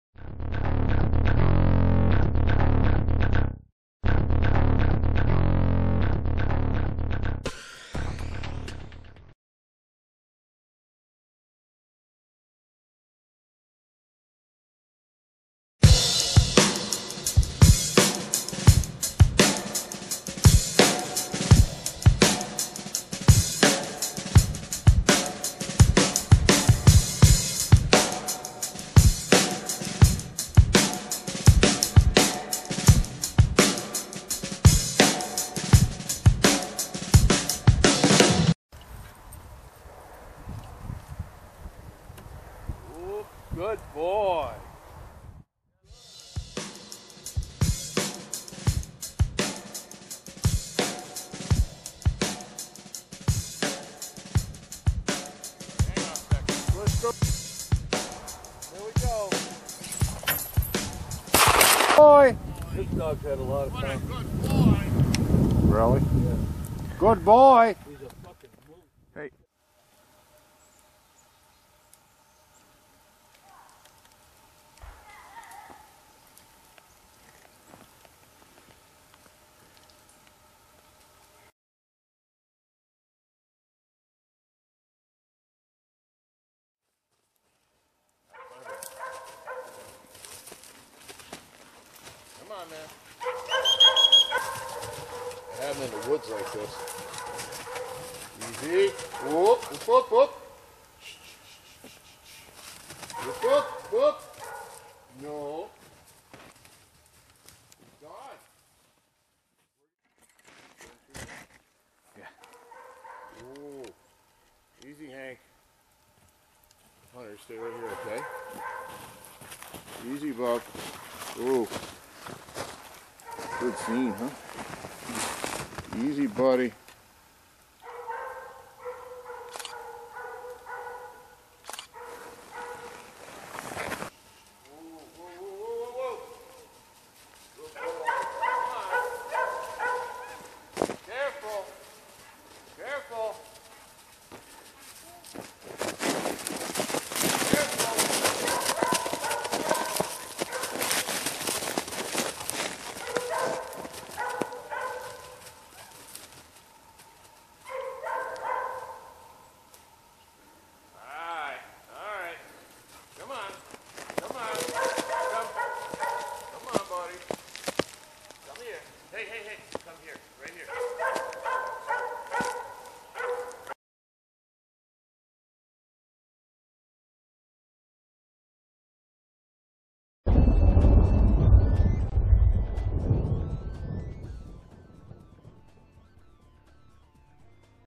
New York Hunting Dog For Sale-Lightning Flash English Setters
One year old Started Hunting Dog for sale. Deeply rooted in Grouse Ridge Setter breeding, Lightning Flash Hank is a loving dog, and an excellent hunter. or email for more information.